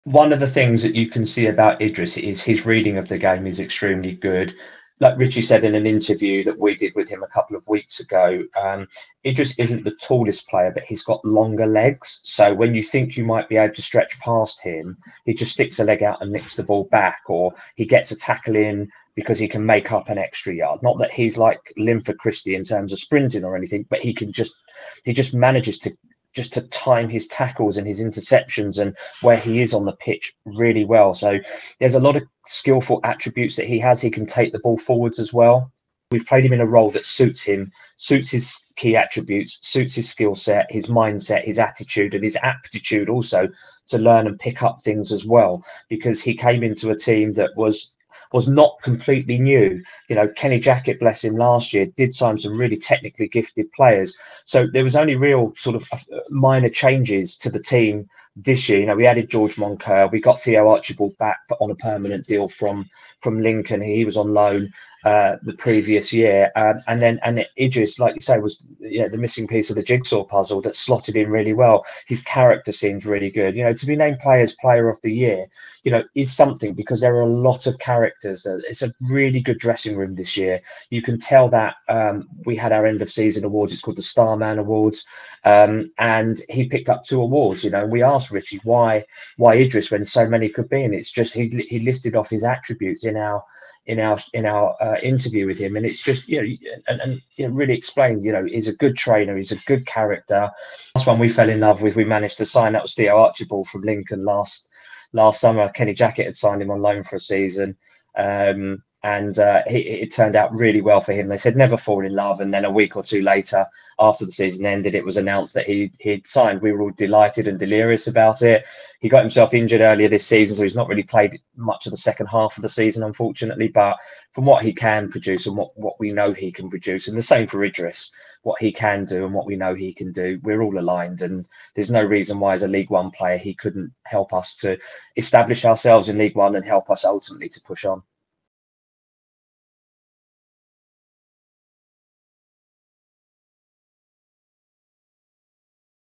Insight on Idris El Mizouni season loan at Leyton Orient
Ross Halls was joined by Paul from the Orient Outlook Podcast to get some insight on how Idris El Mizouni got on at Leyton Orient in League Two